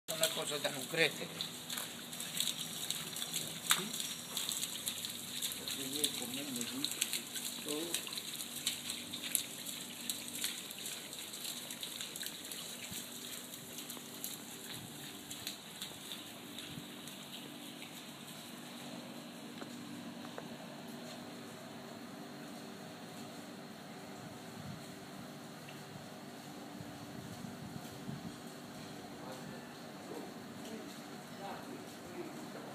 Un fiume d'acqua dalla Cattedrale
Da due giorni una perdita di acqua presumibilmente potabile dall'immobile che fiancheggia la chiesa di Santa Maria ad Alghero. Il fiume si perde in Piazza Civica.